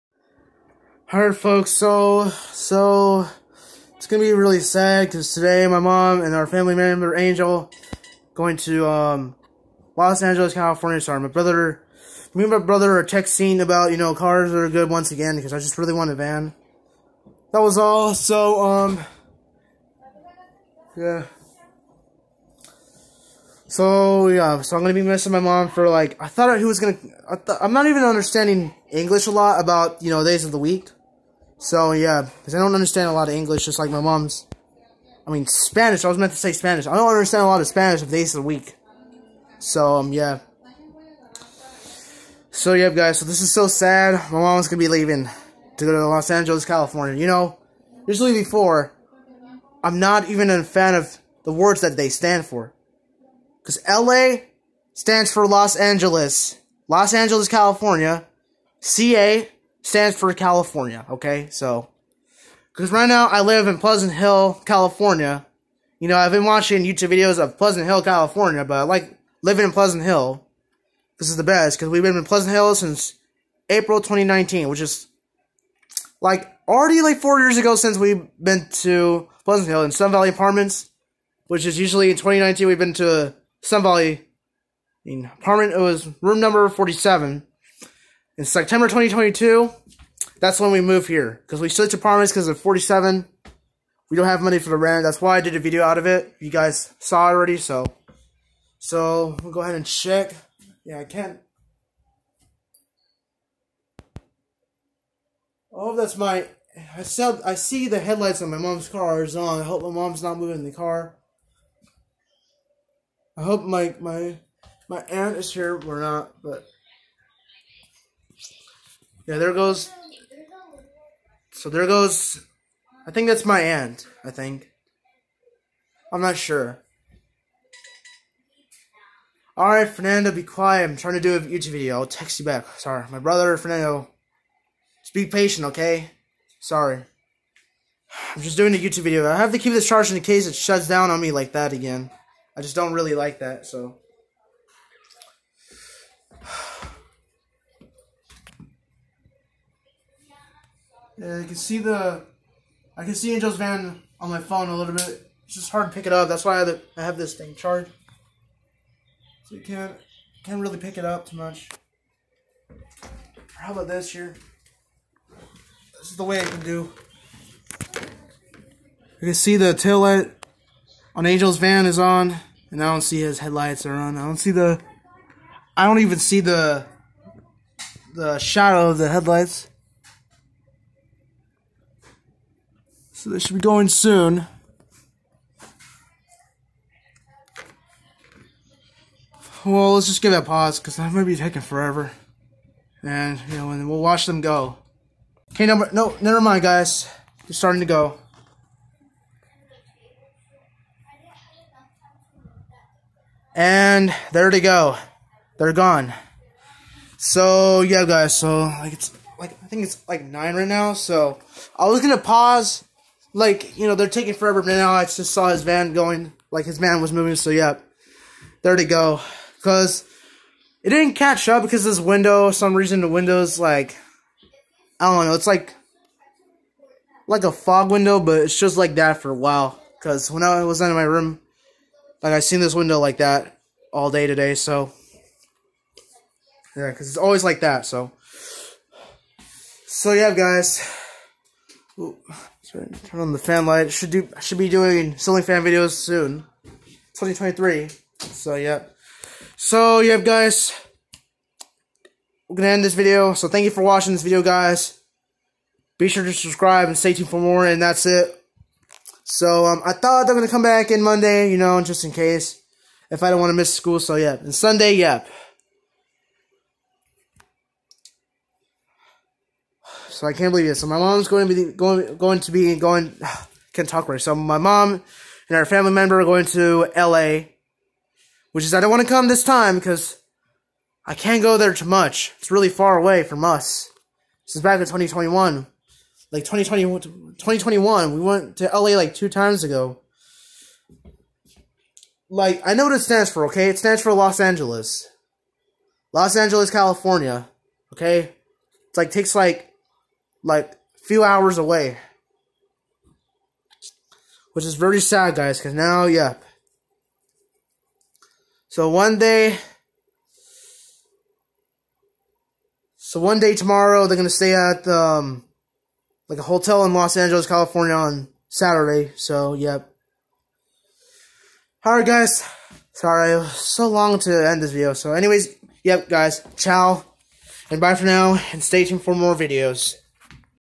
A little sad 😭 news
Today my mom and our family remember angel is going to Los Angeles CA for only 2 days, wich is Sunday they will come back, but I am lucky I get to stay home because my aunt is here, in 2021, we went to Los Angeles CA 2 times ago before I started and stopped playing Pac-Mans, but anyways enjoy and be sure to Subscribe and stay tuned for more videos.